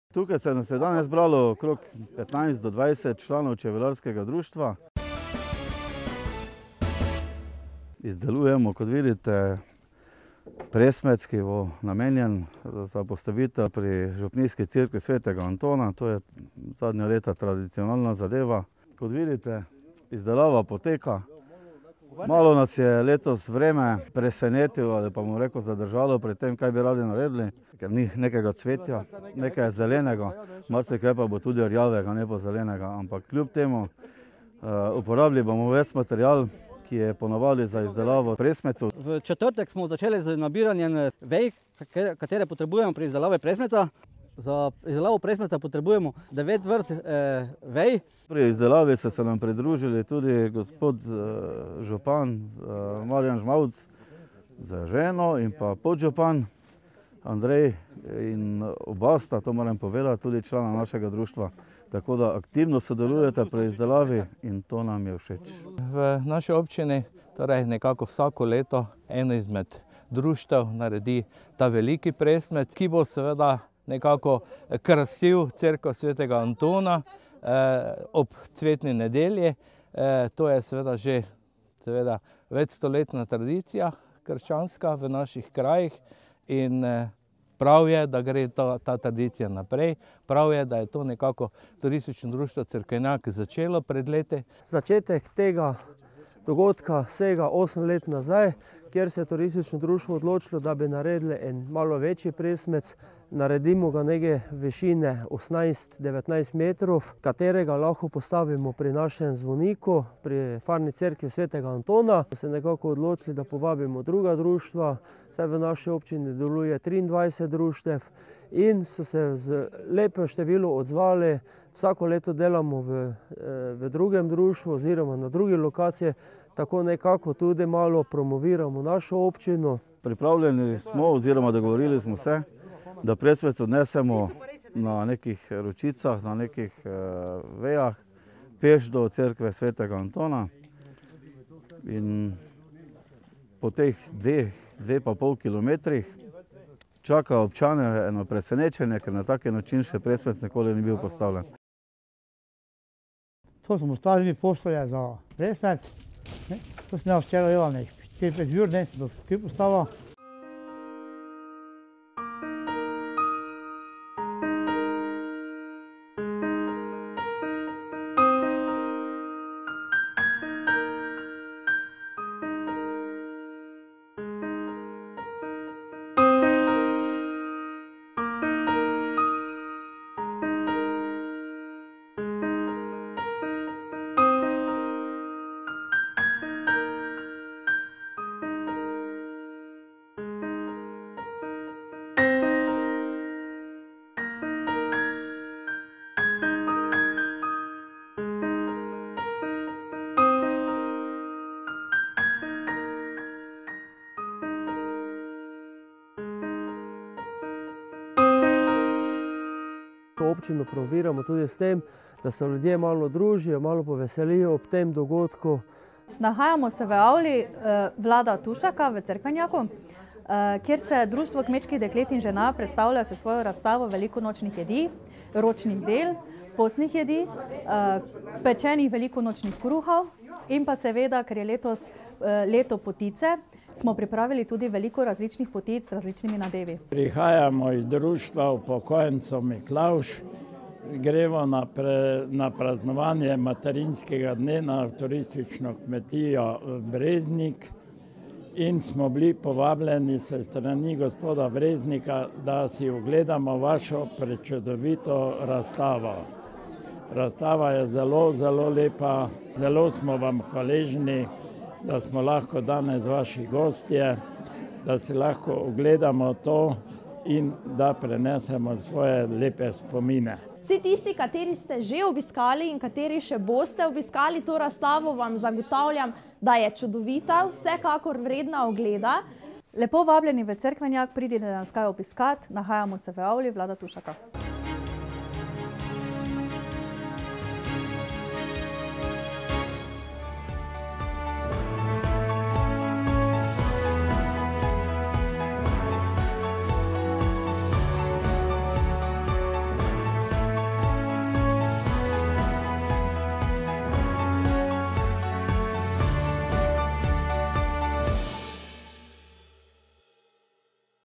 Cerkvenjaški presmec in razstava Velika noč nekoč in danes
Tudi letos so v Cerkvenjaku izdelali in ob cerkev Sv. Antona, na cvetno nedeljo, postavili velik presmec. Izdelali so ga člani Čebelarskega društva Ivan Jurančič Cerkvenjak.

Članice Društva Kmečkih žena in deklet Cerkvenjak ter učenci OŠ Cerkvenjak - Vitomarci pa so, v avli vlada Tušaka, pripravili razstavo Velika noč nekoč in danes.